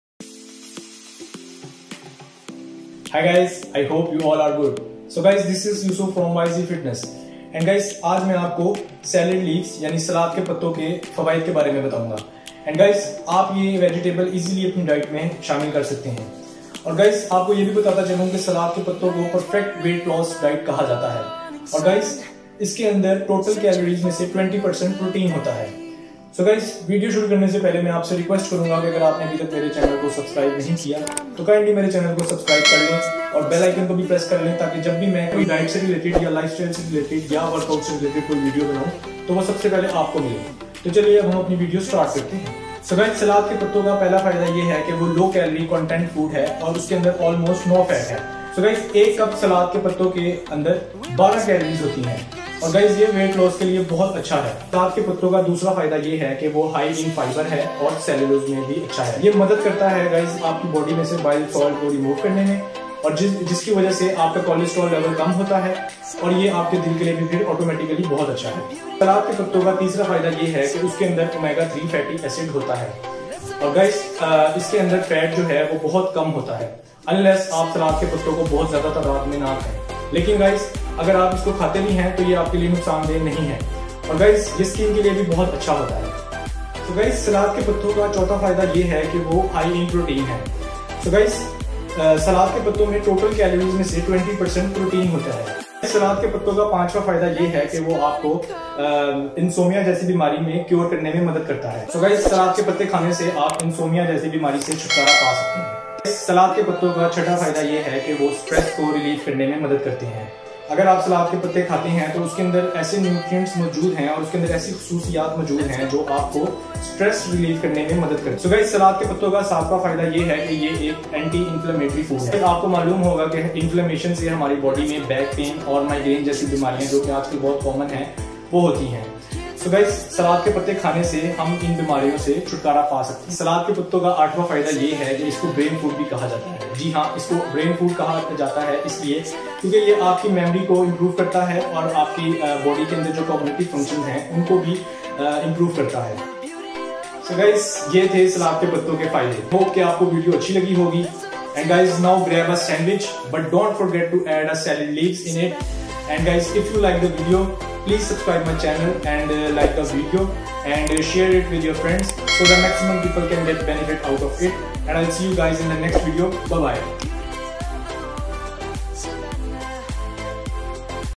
Health Benefits of Lettuce/Salad Leaves | 8 Wonderful Reasons To Eat Salad Leaves| YZ FITNESS| URDU
Hi Guys,
In this video, I will tell you the Health Benefits of Lettuce/Salad Leaves and 8 Wonderful Reasons To Eat Salad Leaves.